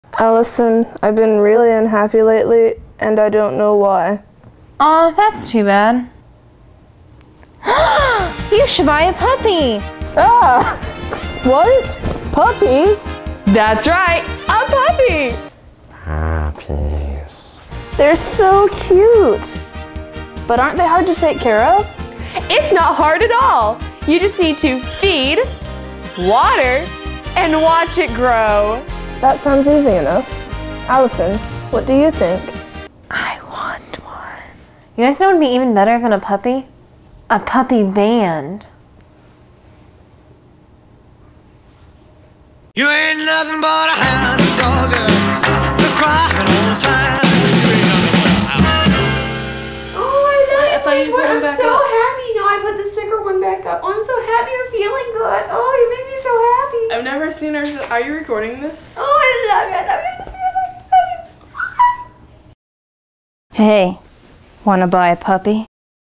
Puppies for Sale!
Written by and Starring
Allison Miller
Allison Mopps
and Sarah Wilks

with Tabitha Seid as The Creepy Lady

Directed, Shot, and Edited by Neal Werle (who loved every minute of it!)

Special thanks to the Mopps household, especially Maddie, for all their help and puppies.